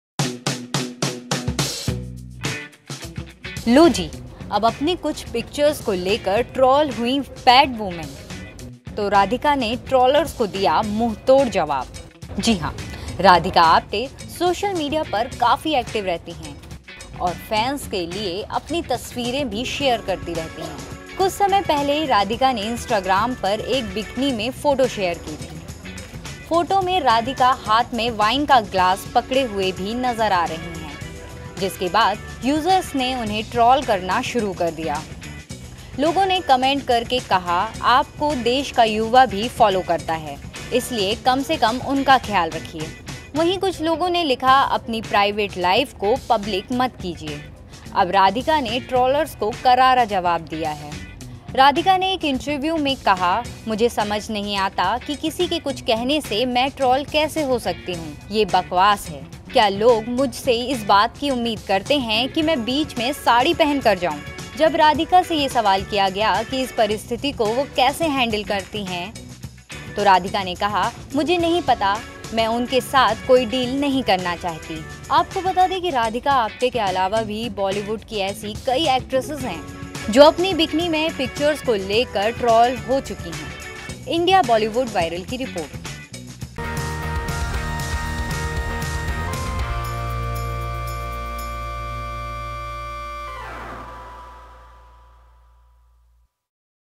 बिकनी में फोटो को लेकर ट्रोल हुईं राधिका, टोर्ल्स को दिया करारा जवाब...
अपनी कुछ तस्वीरों को लेकर ट्रोल हुईं राधिका आप्टे, तो टोर्ल्स को दिया मुंहतोड़ जवाब... देखिए ये रिपोर्ट...